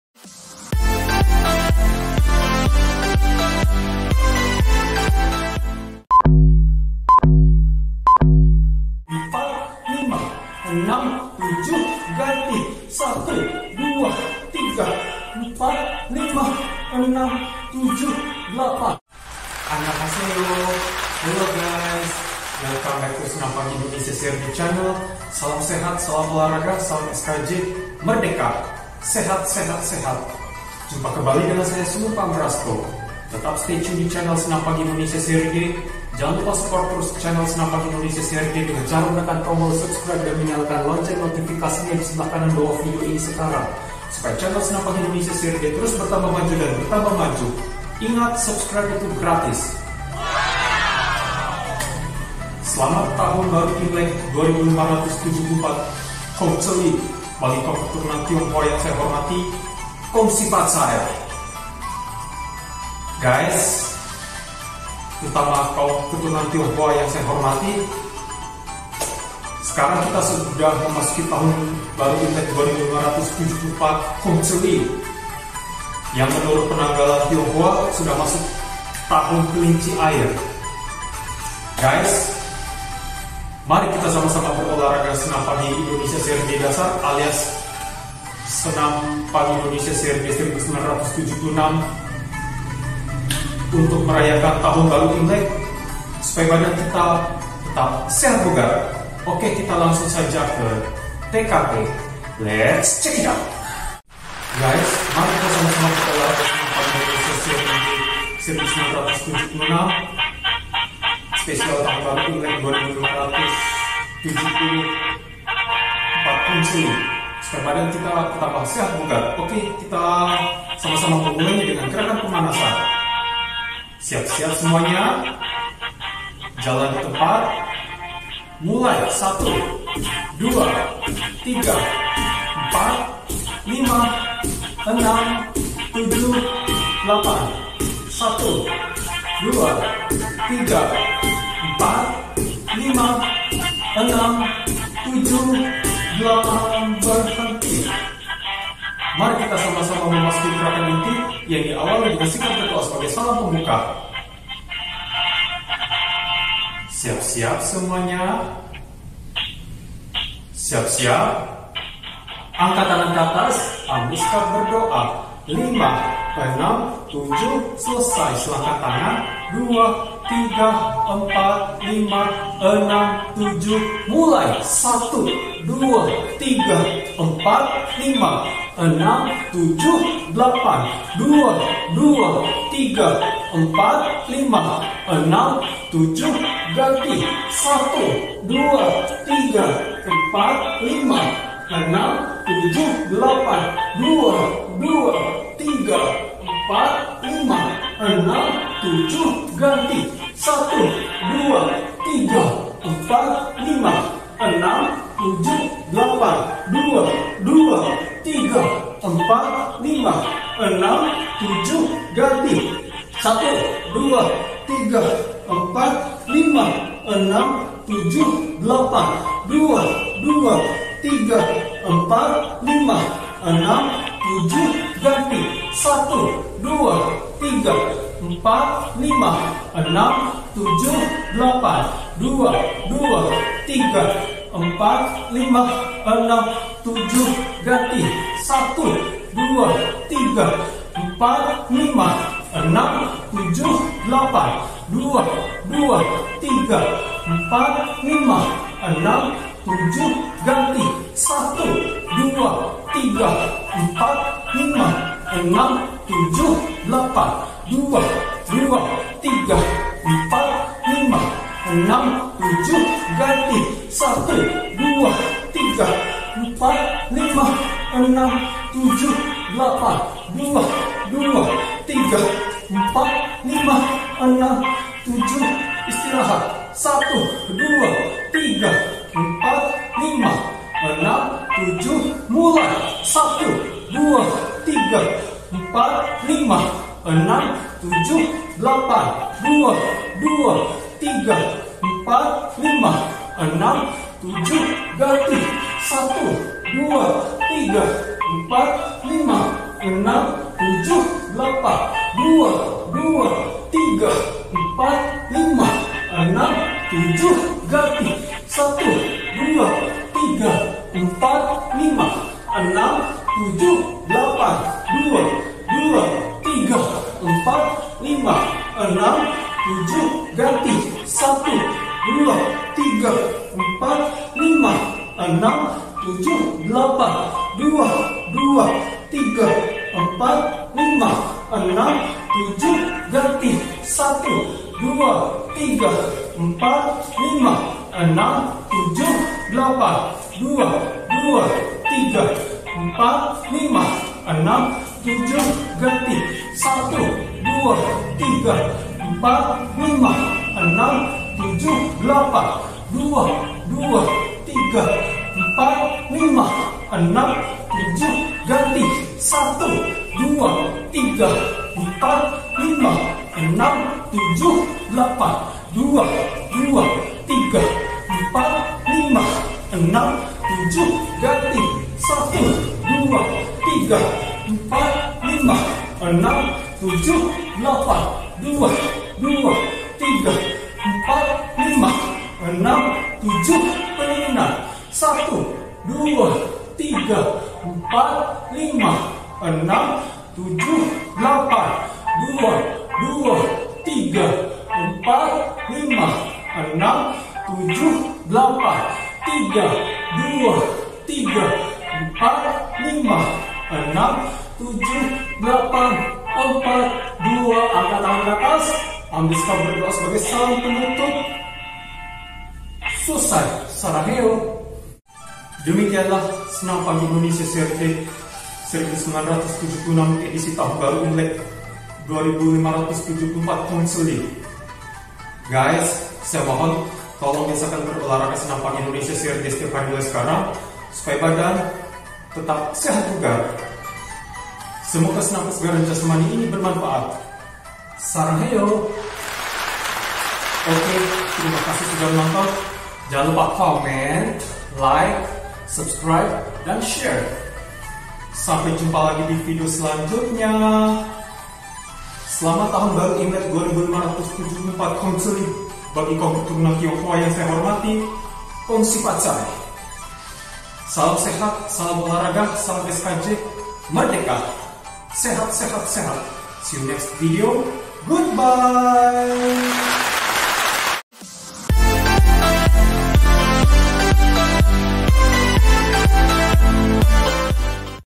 VIRALKAN! SENAM PAGI INDONESIA SERI D 1976 EDISI TAHUN BARU IMLEK 2574 KONGZILI
Judul: VIRALKAN! SENAM PAGI INDONESIA SERI D 1976 EDISI TAHUN BARU IMLEK 2574 KONGZILI

Salam SKJ! 💪
2574孔子里新年快乐。 中国新年快乐。Selamat Tahun Baru Imlek 2574 Kongzili bagi kaum keturunan Tionghoa yang saya hormati. Gong Xi Fa Cai...! Sekarang kita sudah memasuki tahun kelinci air menurut penanggalan Tionghoa. Mari kita rayakan Tahun Baru Imlek 2574 Kongzili dengan berolahraga Senam Pagi Indonesia Seri D 1976, supaya badan kita bertambah sehat bugar. Semoga bermanfaat...! 😀🙏

setelah selesai menonton.

Salam SKJ, 💪
SULUNG PAMBRASTO